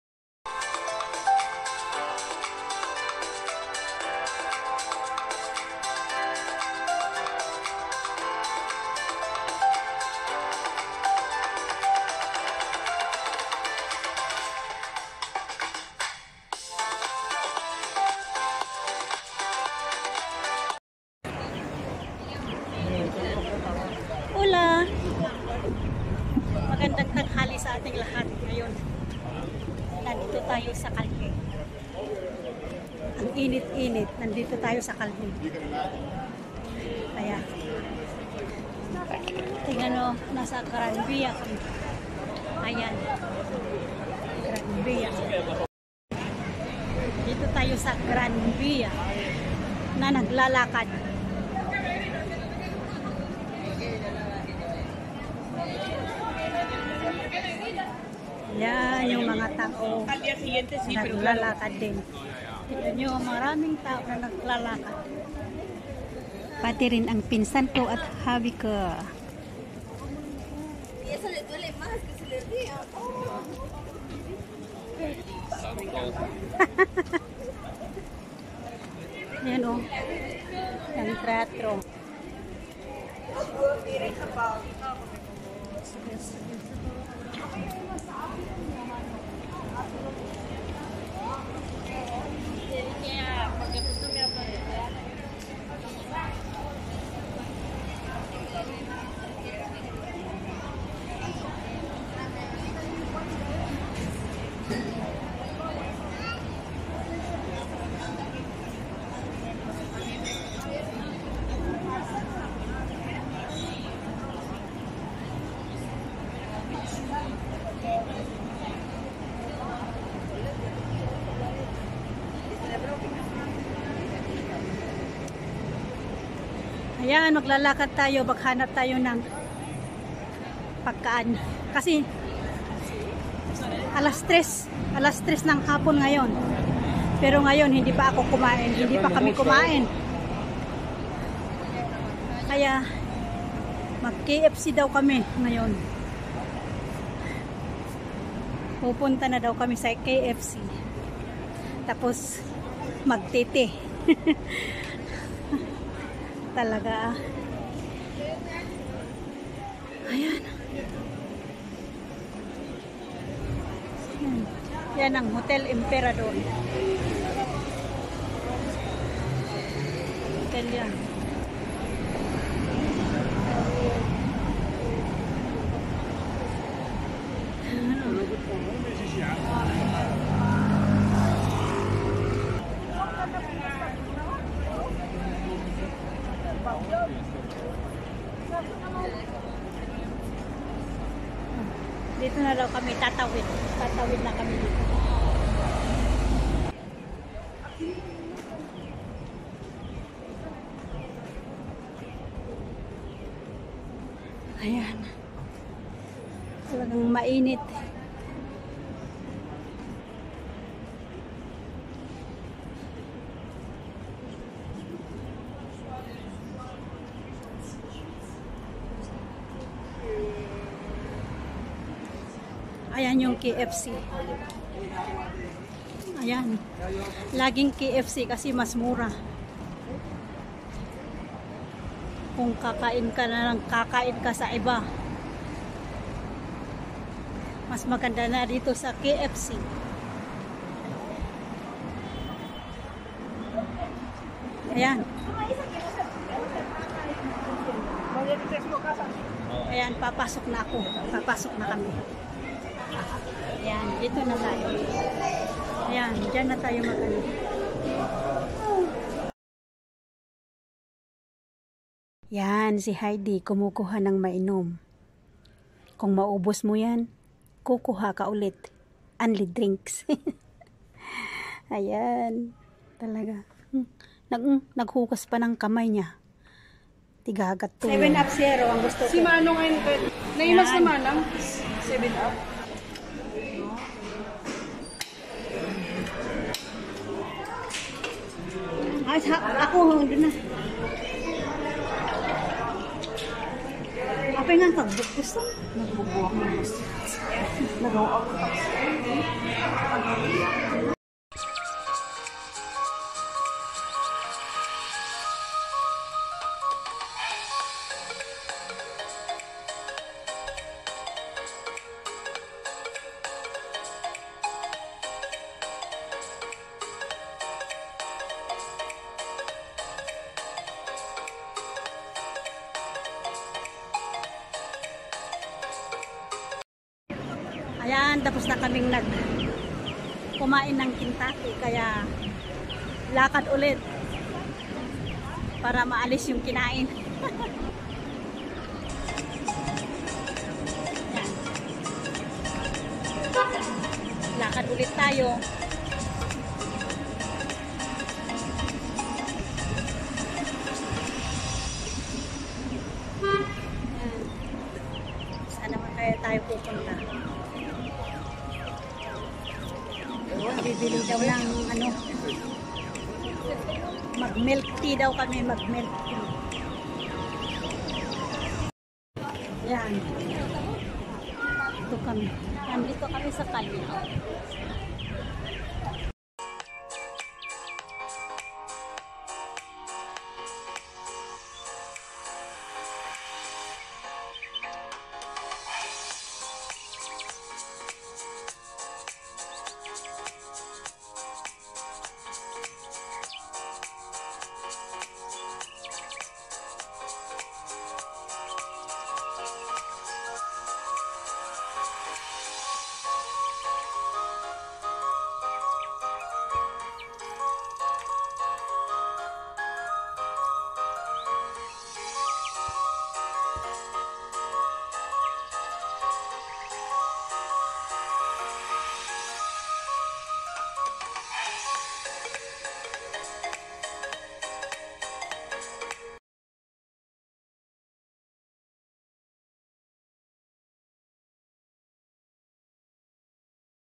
Naglakad at Kumain sa KFC
Naglakad kami at kumain ng manok sa kfc.